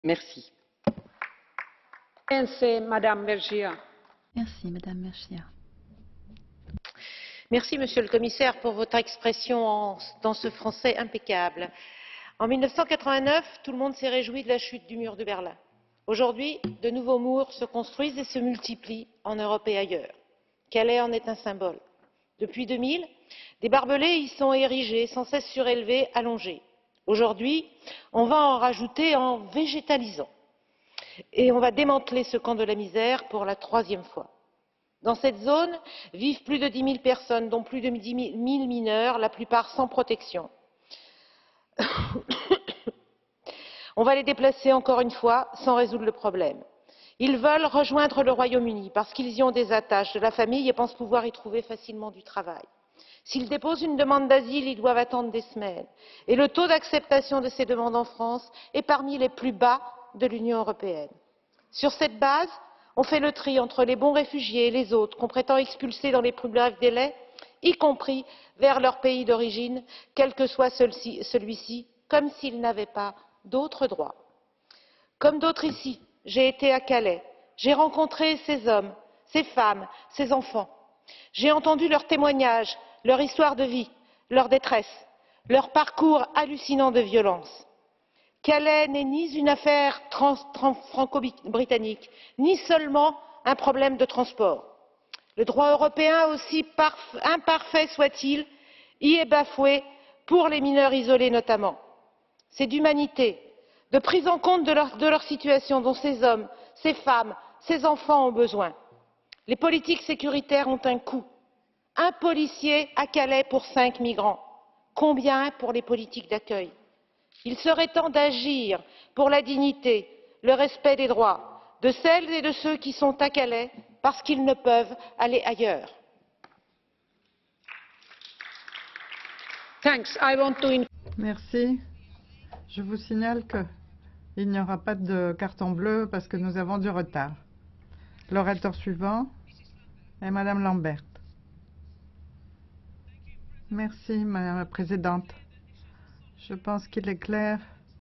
Mon intervention sur la situation à Calais
Strasbourg, le mardi 4 Octobre 2016 - Je suis intervenue en séance plénière sur la décision de démantèlement du camp de réfugiés de Calais.